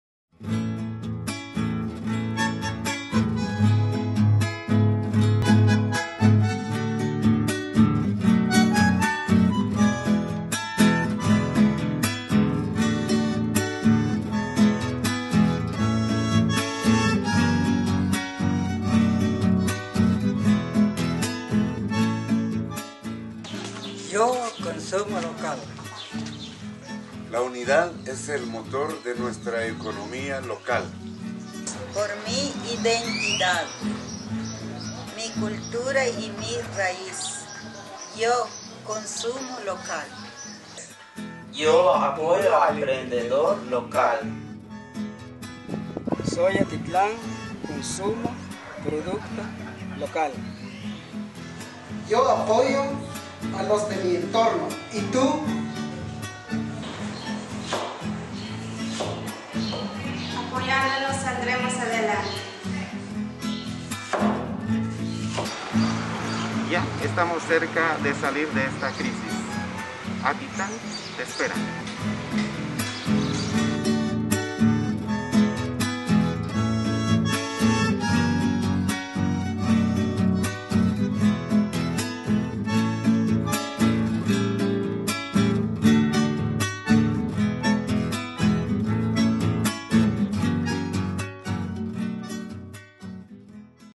Atitlán Somos Todos, Consumo Local.
Mensaje de solidaridad y unidad ante la pandemia. Atitlánconcriterio.